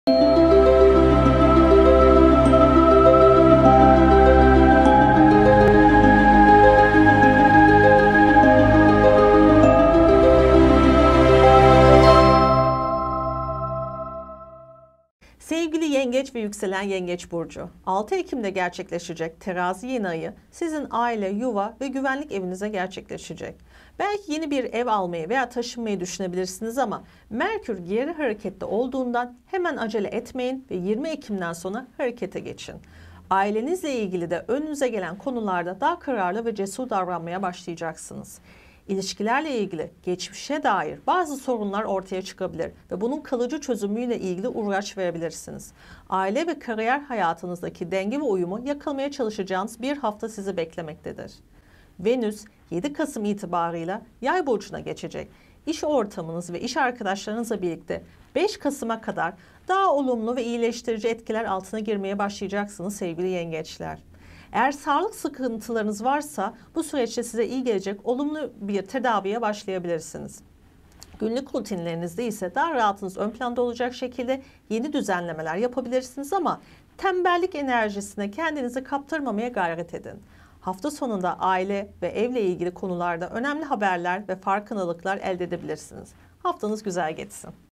4-10 Ekim Yengeç Burcu Yorumu
Bu hafta 6 Ekim günü Terazi Yeniayı gerçekleşecek. Yeniay etkileri, diğer gökyüzü hareketlerinin genel ve burçlar üzerindeki etkisi nasıl olacak ‘’ Astrolog Yasemin Sürer ile Yıldızların Dili ‘’ programında yeni haftalık videosunda anlattı.

Günlük-haftalık astrolojik bilgiler, gezegen hareketleri ve astrolojik tüm öngörüleri için Astrolog Yasemin Sürer’ i takip edin.